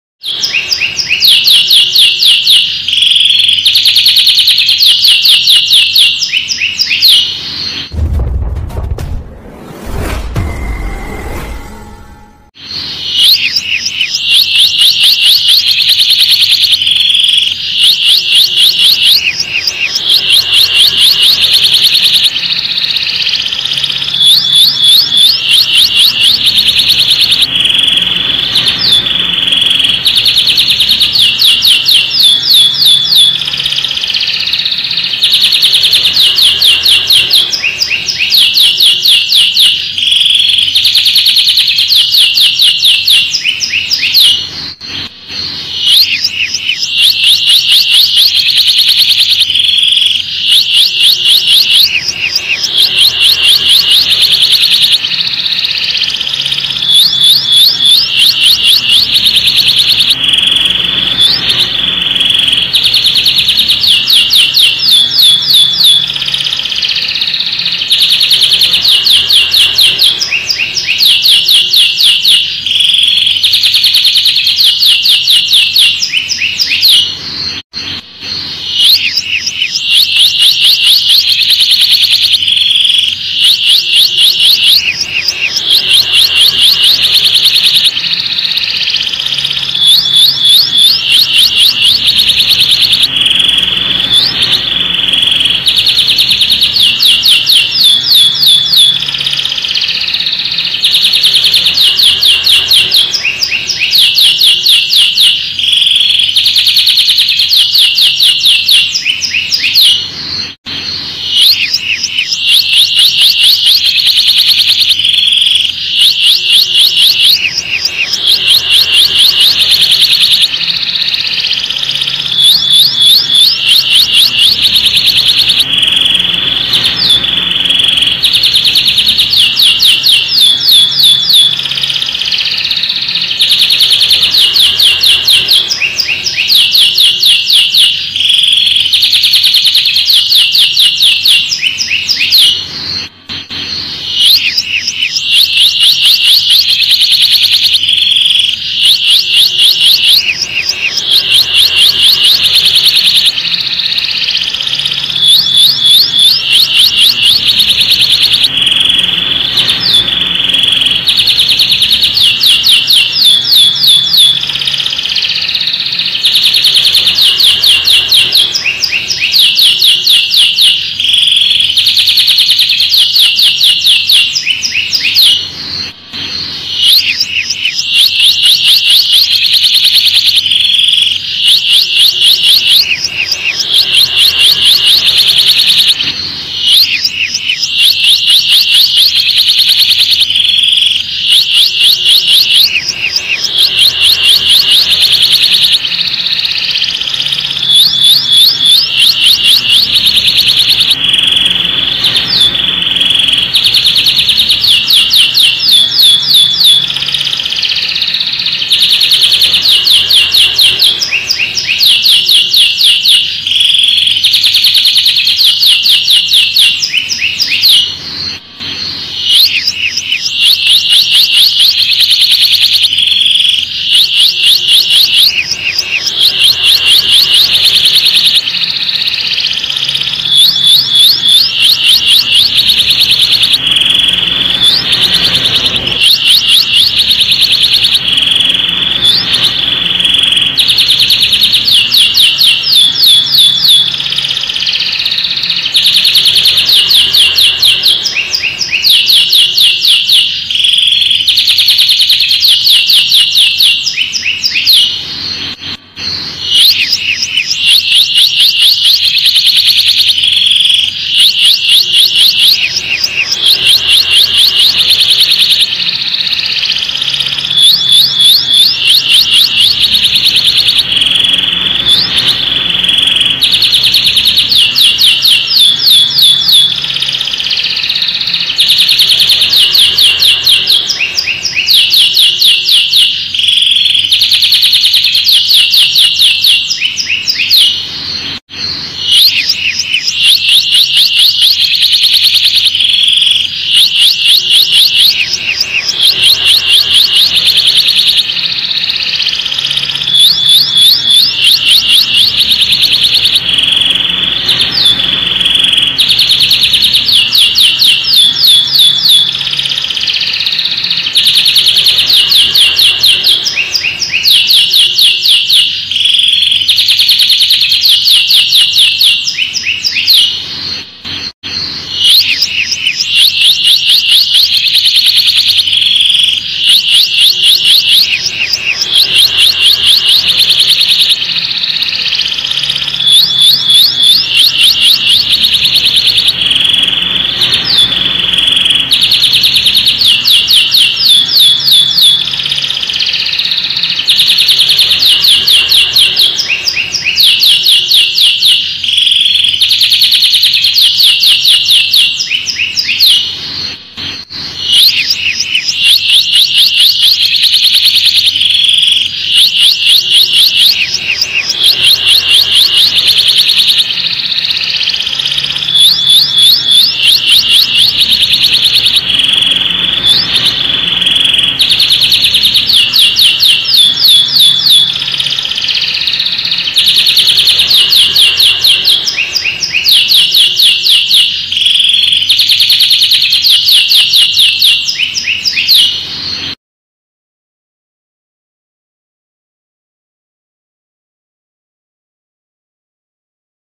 KICAU KENARI UNIK Untuk MASTERAN KENARI BAHAN DAN PAUD Belajar Bunyi
Kicau Kenari unik gacor untuk masteran supaya kenari paud cepat bunyi dan supaya mempunyai lagu dan cengkok terbaik,semoga panciingan ini cocok dengan kenari temen temen...trima kasih semoga bermanfaat